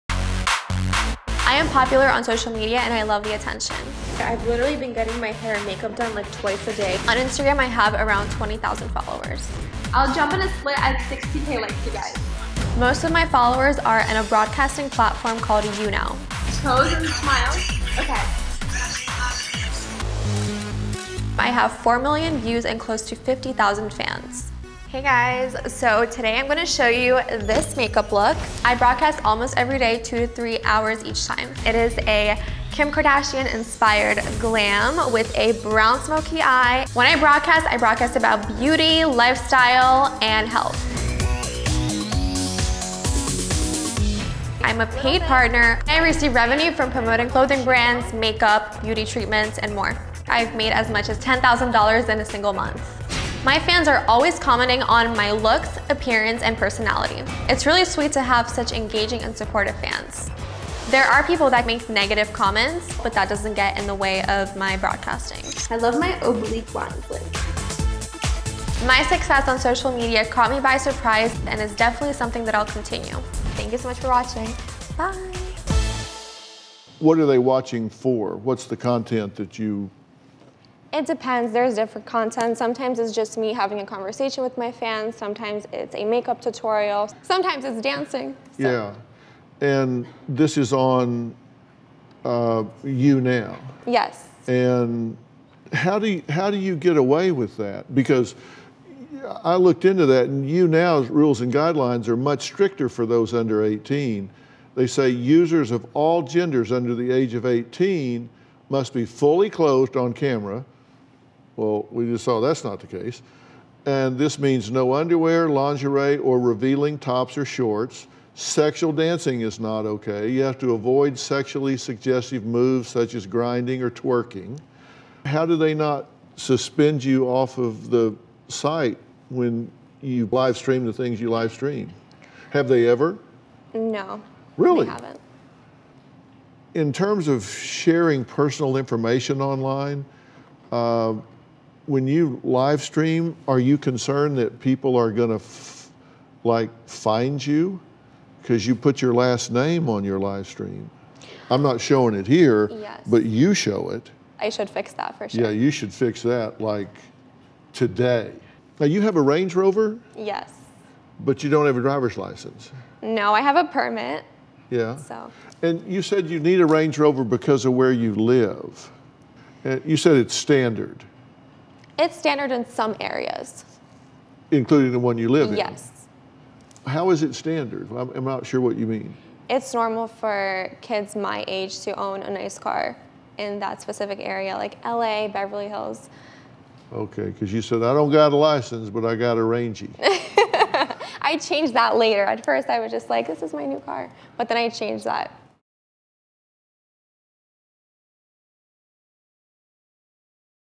'I Am Popular On Social Media And I Love The Attention,' Says 17-Year-Old
A teen who broadcasts on social media almost every day says she “loves” the attention.